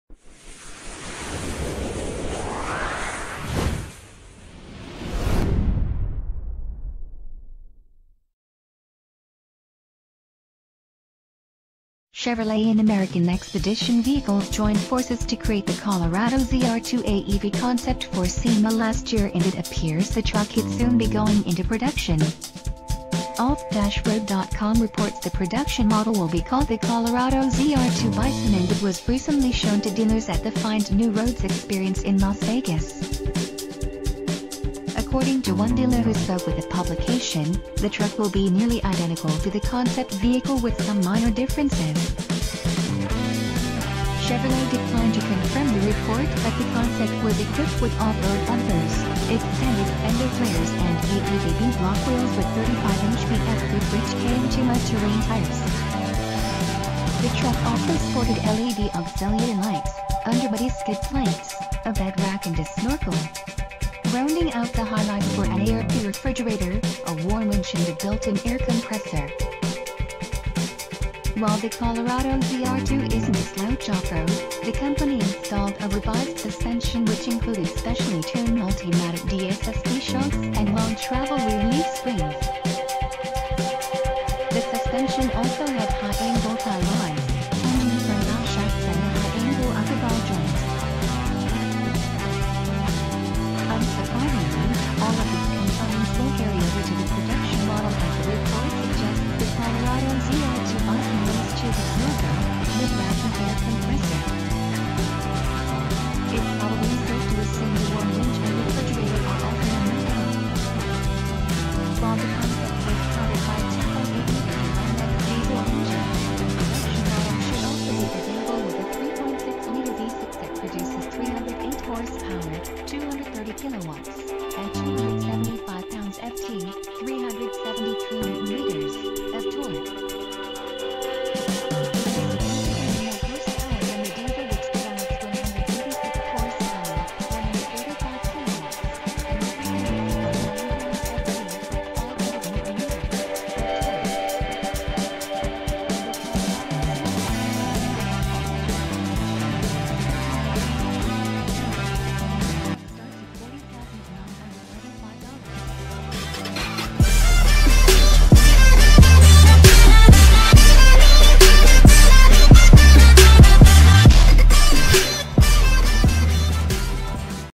Report - Chevrolet Colorado ZR2 Bison Coming To Tackle The Tacoma TRD Pro.
Chevrolet and American Expedition Vehicles joined forces to create the Colorado ZR2 AEV concept for SEMA last year and it appears the truck could soon be going into production.

Off-Road.com reports the production model will be called the Colorado ZR2 Bison and it was recently shown to dealers at the Find New Roads Experience in Las Vegas. According to one dealer who spoke with the publication, the truck will be “nearly identical to the concept vehicle with some minor differences.”

Chevrolet declined to confirm the report but the concept was equipped with off-road bumpers, extended fender flares and AEV beadlock wheels with 35-inch BF Goodrich KM2 Mud-Terrain tires. The truck also sported LED auxiliary lights, underbody skid plates, a bed rack and a snorkel. Rounding out the highlights were an ARB refrigerator, a Warn winch and a built-in air compressor.

While the Colorado ZR2 isn’t a slouch off-road, the company installed a revised suspension which included specially tuned Multimatic DSSV shocks and long-travel rear leaf springs. The suspension also had high-angle tie rods, plunging front half-shafts and a high-angle upper ball joint.

Unsurprisingly, all of these components won’t carry over to the production model as the report suggests the Colorado ZR2 Bison will eschew the snorkel, bed rack and air compressor. It’s probably safe to assume the Warn winch and refrigerator are also a no-go.

While the concept was powered by a 2.8-liter Duramax diesel engine, the production model should also be available with a 3.6-liter V6 that produces 308 hp (230 kW) and 275 lb-ft (373 Nm) of torque. It has significantly more horsepower than the diesel which develops 186 hp (135 kW) and 369 lb-ft (500 Nm) of torque.

The Colorado ZR2 Bison is expected to arrive in showrooms shortly but it will likely be expensive as the standard truck starts at $40,995.